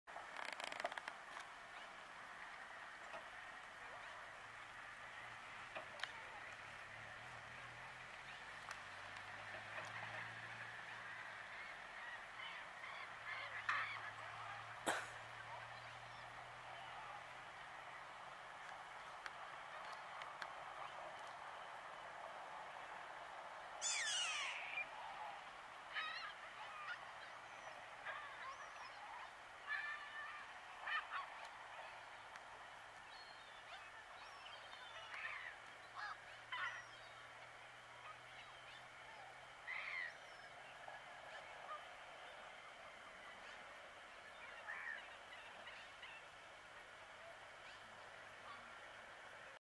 Aves playeras en Caulín(5)
garzas, huairavos, gaviotas y zarapitos